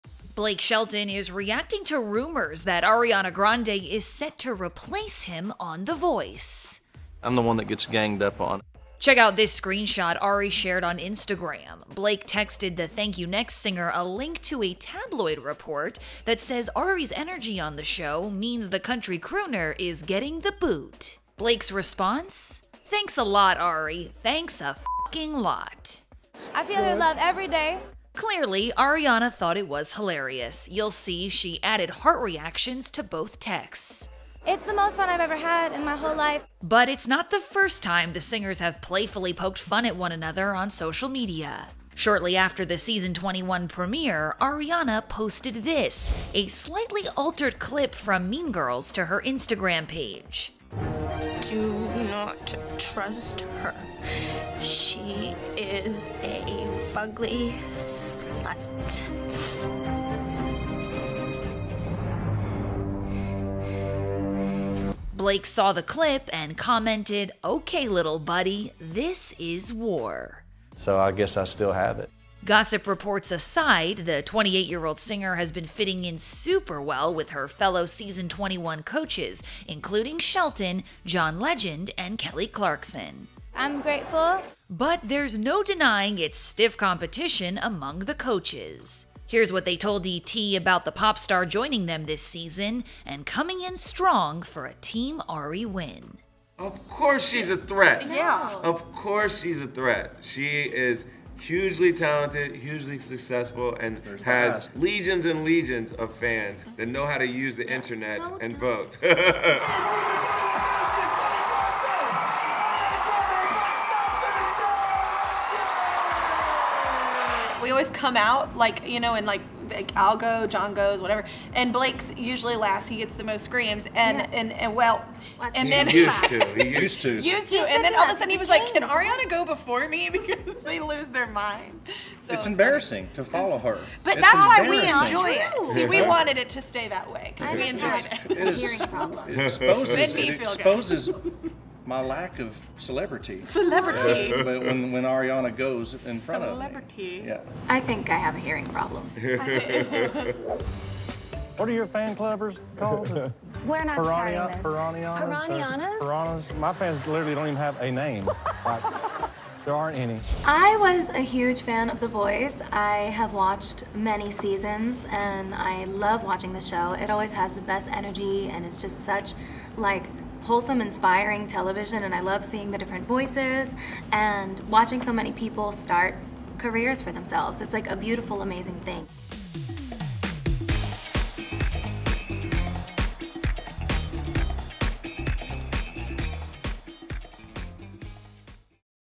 Ariana Grande REPLACING Blake Shelton on The Voice?!
Ariana Grande shared a screenshot of her text messages with Blake Shelton, reacting to a report that she’s replacing him on ‘The Voice.’